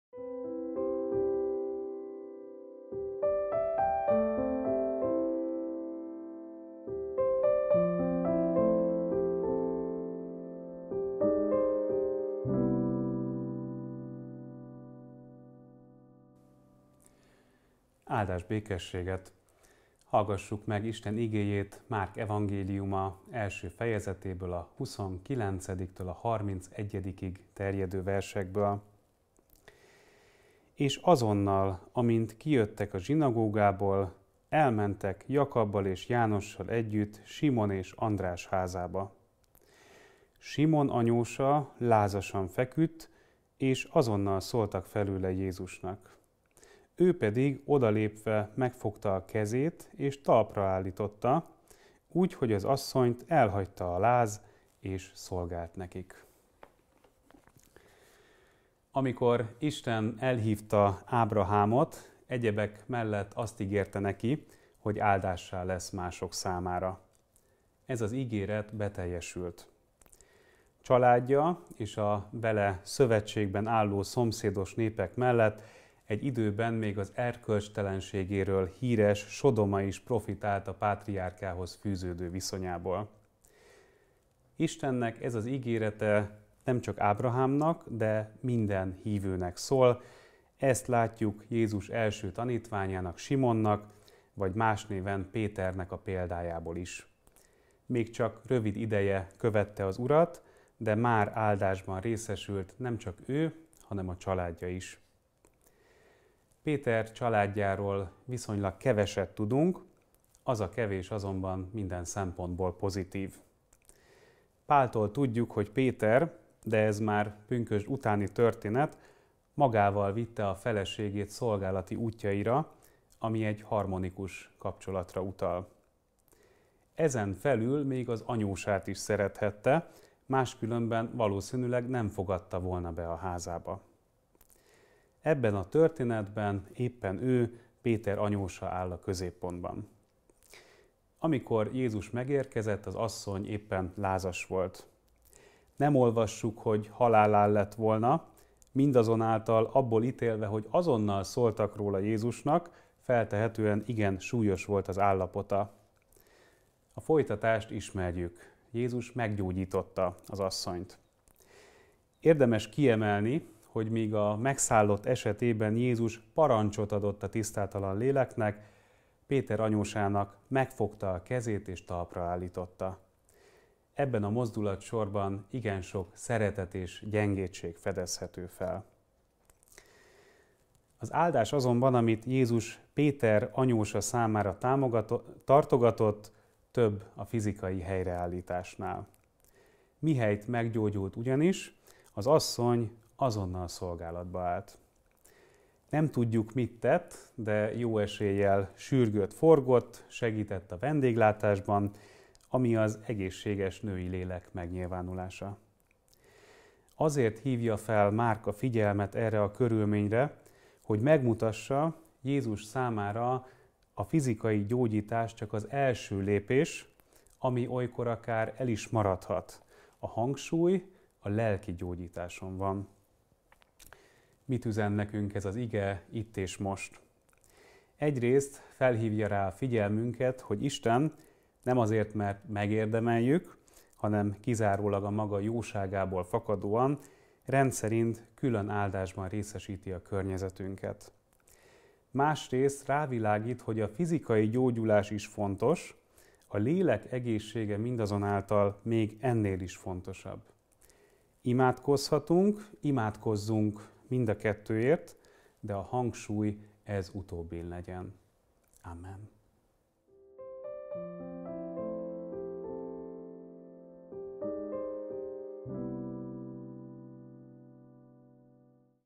Igeidő - 2023.04.20.
A zsinagógából Jakabbal és Jánossal együtt egyenesen Simon és András házába ment. Simon anyósa lázas betegen feküdt. Ezt azonnal elmondták Jézusnak. Odament hát hozzá, s kézen fogva fölsegítette. Erre megszűnt a láza, és szolgált nekik. (Márk evangéliuma 1. fejezet 29-31 vers)
Napindító áhítat Szabó Lászlóval